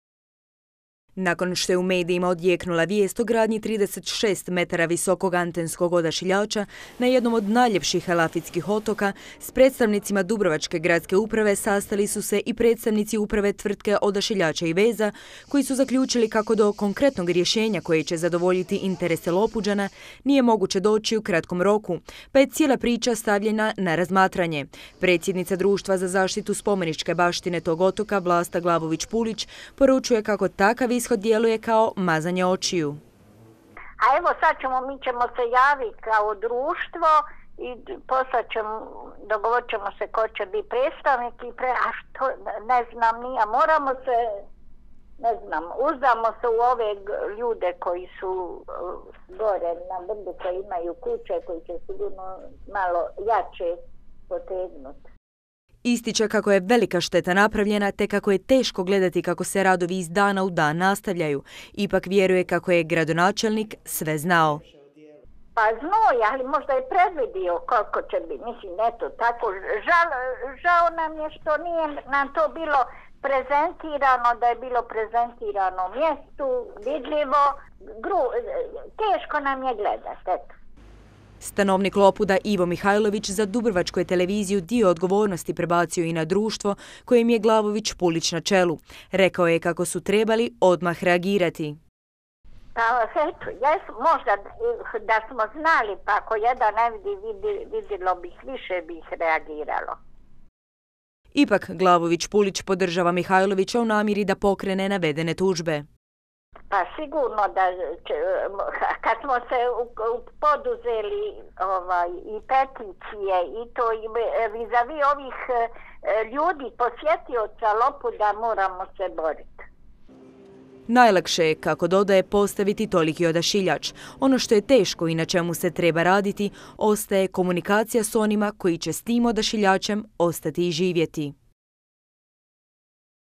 Stigla reakcija na odašiljač iz Društva za zaštitu baštine na Lopudu!
DUTV I dalje pratimo situaciju oko izgradnje 36 metara visokog odašiljača na otoku Lopudu i razgovaramo s otočanima. Predsjednica društva za zaštitu spomeničke baštine tog otoka Vlasta Glavović Pulić otvoreno govori kako je velika šteta napravljena. I ona je uvjerena kako su svi sve znali, a žao joj je što otočanima nitko ništa nije prezentirao.